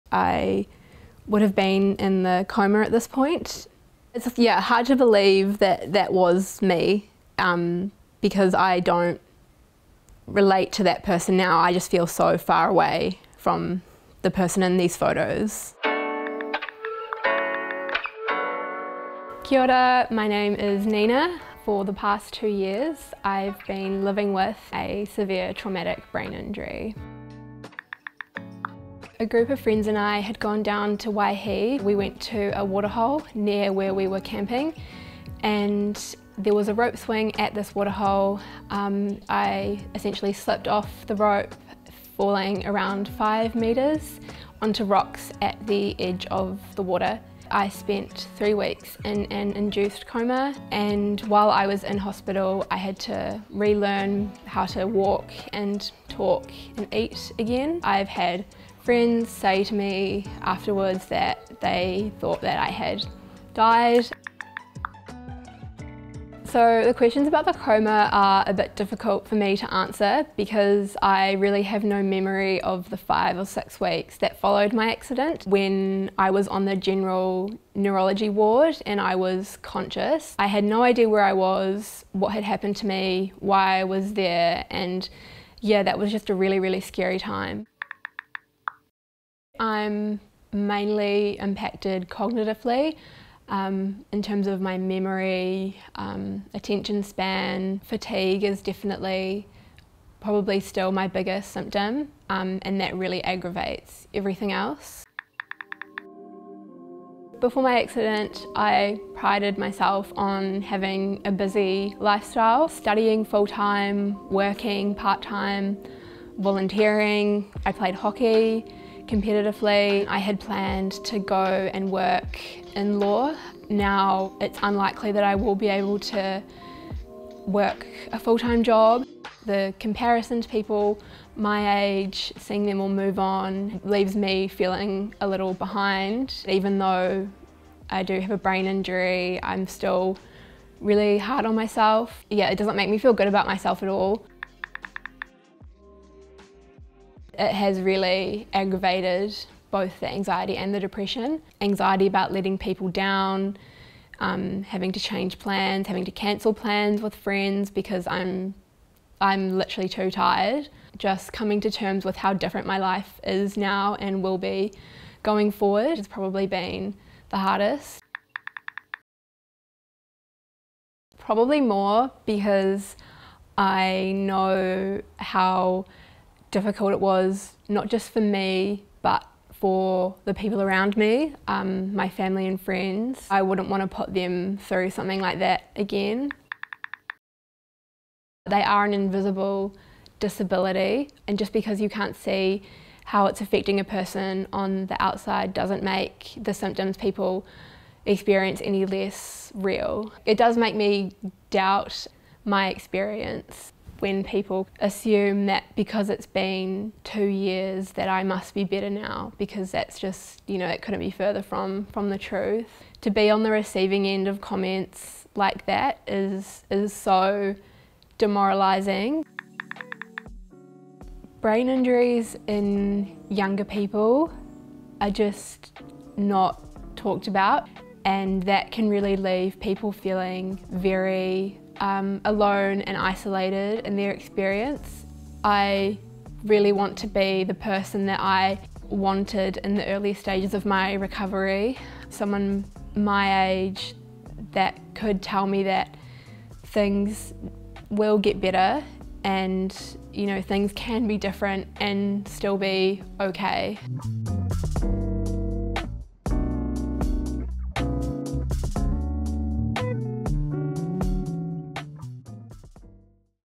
A person with a brain injury answers your questions
Nina Duggan suffered a traumatic brain injury after she slipped off a rope swing two years ago.

She spent three weeks in an induced coma and had to learn how to walk, talk and eat again.

“Even though I do have a brain injury, I'm still really hard on myself. The comparison to people my age seeing them all move on and do adult things does leave me feeling behind. It doesn't make me feel good about myself at all.”

Re: News journalist @zoemaddensmith sat down with Nina to talk about what it is like to live with a brain injury and how there needs to be more awareness for this invisible disability.

This video is part of our series ‘Answering Your Questions’.

Re: makes videos, articles and podcasts about the things that matter to young people in Aotearoa.